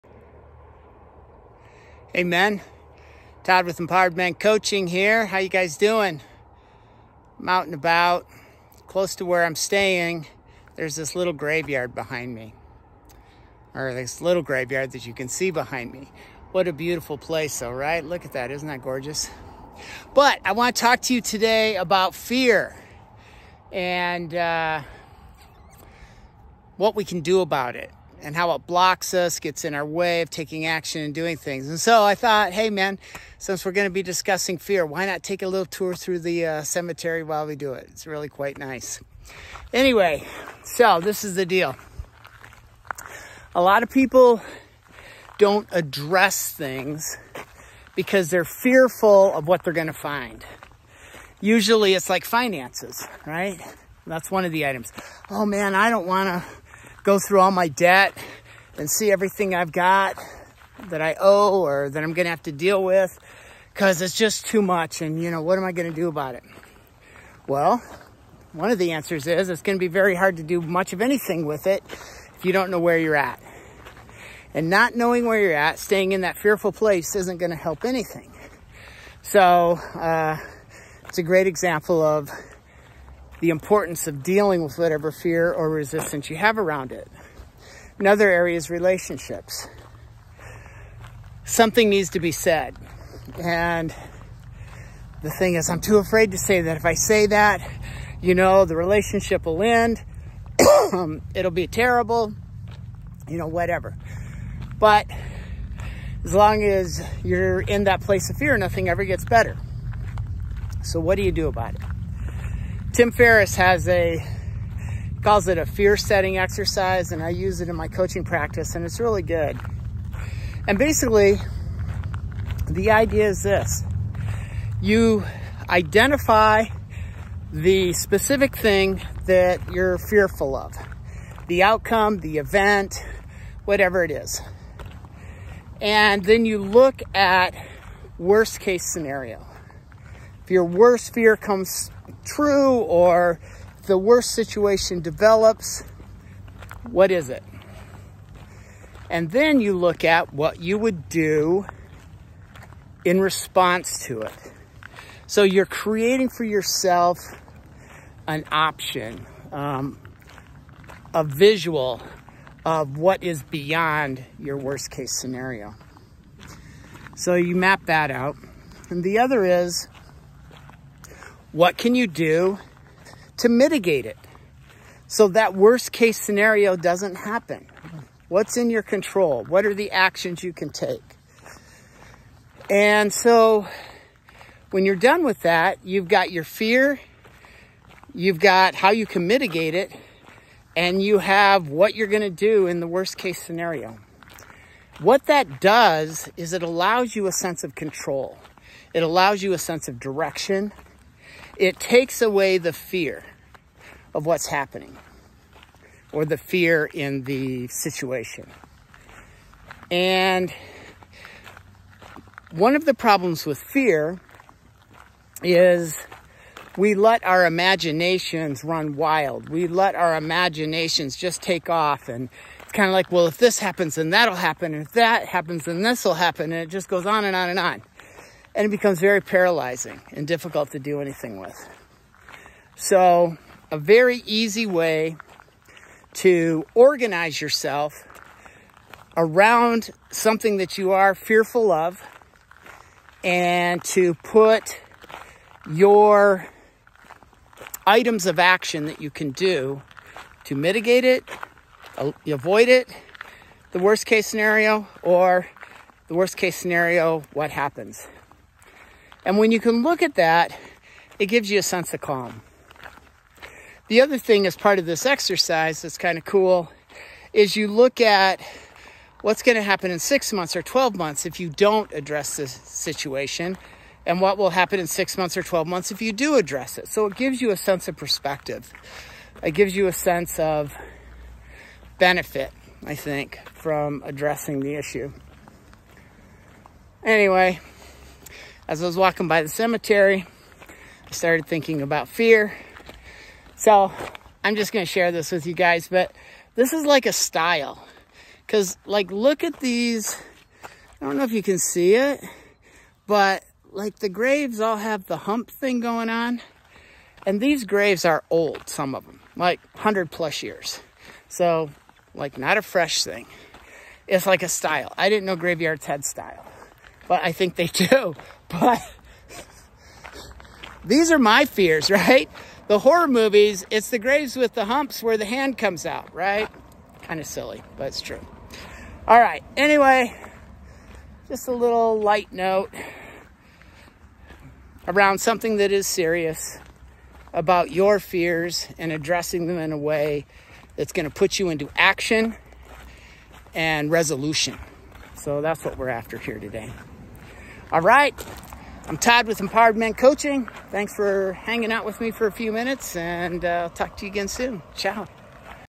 How to Stop Fear from Blocking Your Progress
In this video, Todd from Empowered Man Coaching explores how fear can hold us back and prevent us from taking action in our lives. Using a simple yet powerful exercise called fear setting, Todd shares how to identify and address the root causes of your fears, and how to take control of the situation.

Whether it's facing financial challenges, having difficult conversations in relationships, or tackling any area of life that causes anxiety, fear setting helps you break down the worst-case scenarios, mitigate them, and create a plan for action. This exercise not only helps you overcome fear but also gives you a sense of clarity and control, allowing you to move forward with confidence.